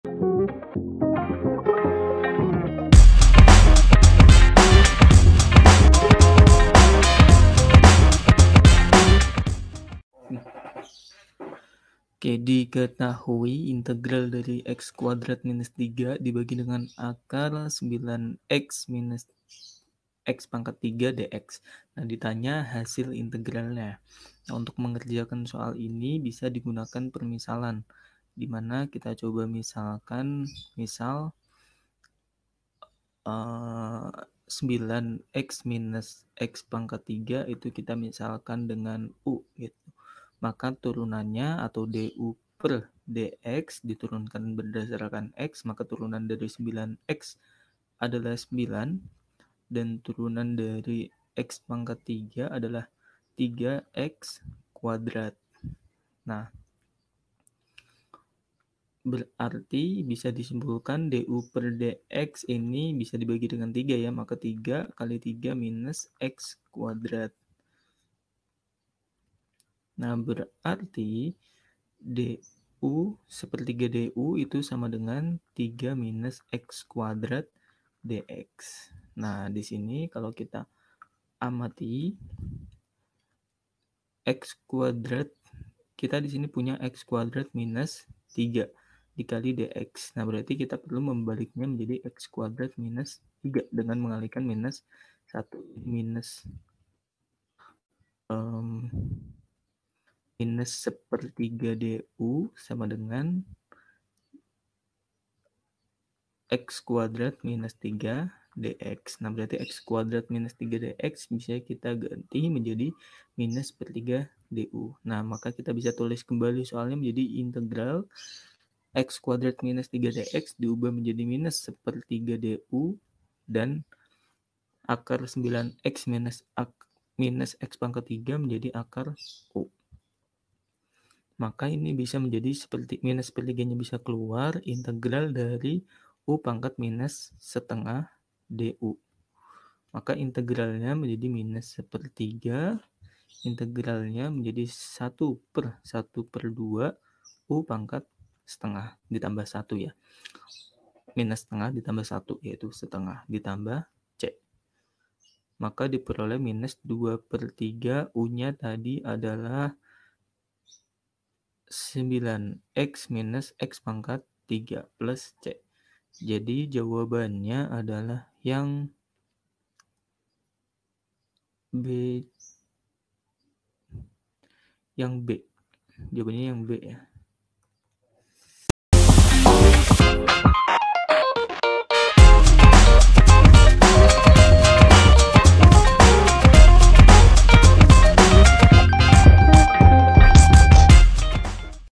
Hasil dari \int \frac{x^2-3}{\sqrt{9x-x^3... | Matematika Untu | Kelas 11 | 8
✅ Dalam video ini kita akan membahas: Hasil dari  \int \frac{x^2-3}{\sqrt{9x-x^3}}dx=\ldots
A.  \frac{2}{3}\sqrt{9x-x^3}+C
B.  \text{-}\frac{2}{3}\sqrt{9x-x^3}+C
C.  \frac{2}{3}x\sqrt{9x-x^3}+C
D.  \text{-}\frac{2}{3}x\sqrt{9x-x^3}+C
E.  \text{-}\frac{2}{3}x^2\sqrt{9x-x^3}+C
Tema:
Kelas: 11
Mata Pelajaran: Matematika
Bab: 8
Buku: Matematika Untuk SMA/MA Kelas XI Kelompok Wajib/2

👉 Buku yang telah tersedia:
{PLAYLIST_LINK}
Di mana kamu dapat menemukan kami?